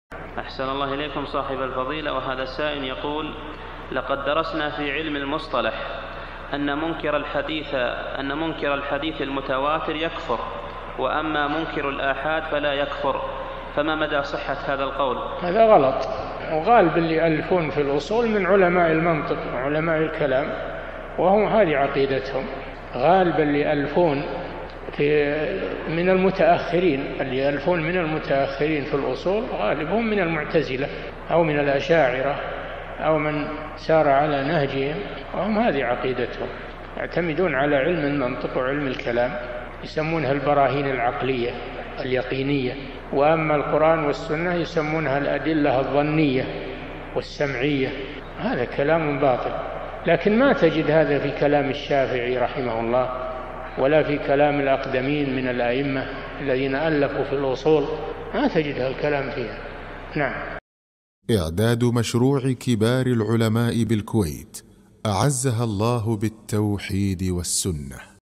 [210 -560] هل صحيح أن منكر الحديث المتواتر يكفر، وأما منكر الآحاد فلا يكفر؟ - الشيخ صالح الفوزان
-"السؤال: لقد درسنا في علم المصطلح أن منكر الحديث المتواتر يكفر، وأما منكر الآحاد فلا يكفر، فما مدى صحة هذا القول؟
الجواب: هذا غلط، وغالب الذين يألفون في الأصول من علماء المنطق وعلماء الكلام وهم هذه عقيدتهم، غالب الذين يألفون من المتأخرين، الذين يألفون من المتأخرين في الأصول غالبهم من المعتزلة أو من الأشاعرة أو من سار على نهجهم وهم هذه عقيدتهم، يعتمدون على علم المنطق وعلم الكلام، يسمونها البراهين العقلية اليقينية، وأما القرآن والسنة يسمونها الأدلة الظنية والسمعية، هذا كلام باطل، لكن ما تجد هذا في كلام الشافعي رحمه الله، ولا في كلام الأقدمين من الأئمة الذين ألفوا في الأصول، ما تجد هذا الكلام فيها.
للاشتراك في قناة الشيخ صالح الفوزان
سؤال وجواب من شرح السنة للبربهاري ( 560 ) مقطع

للاستفسار والإقتراحات والدعم
رئيس الفريق
لمشروع كبار العلماء